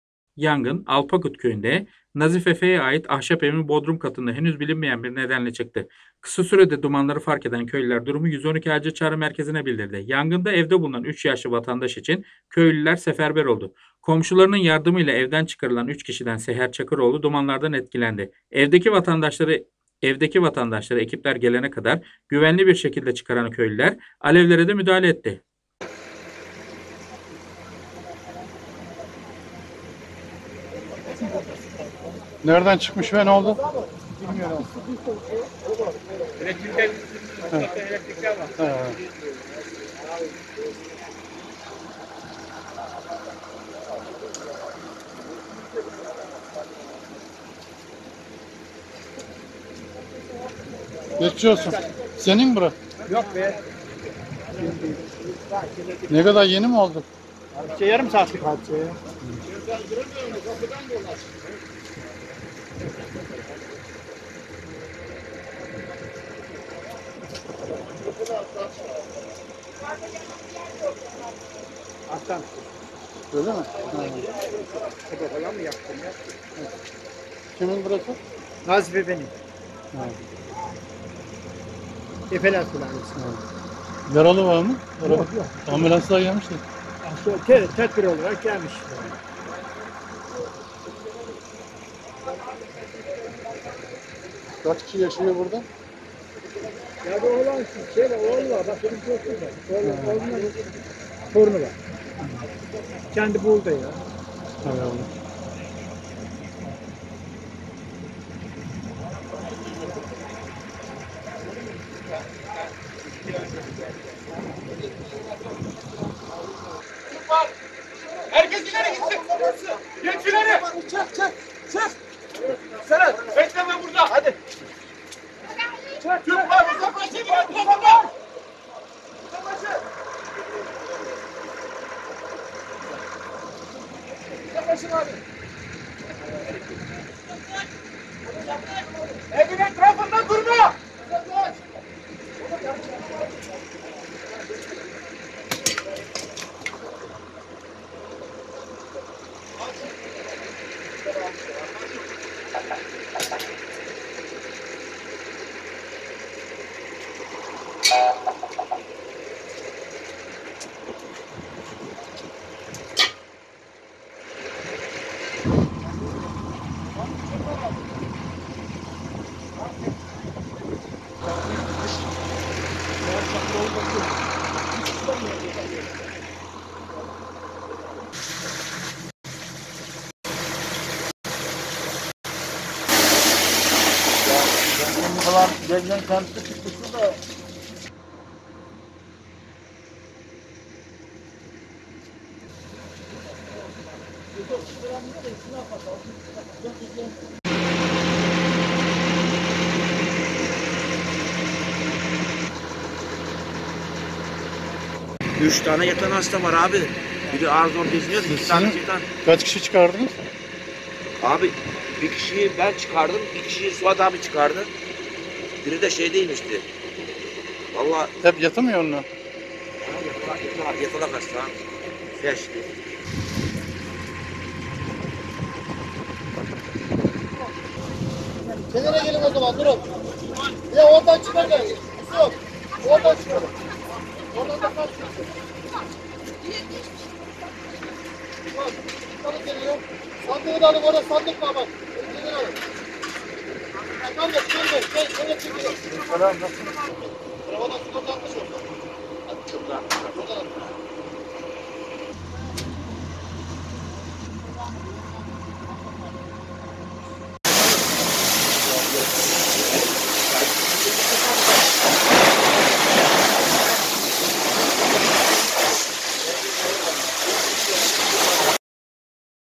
Yangınında Köylüler Seferber Oldu, 3 Kişi Evden Kurtarıldı
Bolu’nun Mudurnu ilçesinde ahşap evde çıkan yangında 3 yaşlı vatandaş köylüler tarafından kurtarıldı.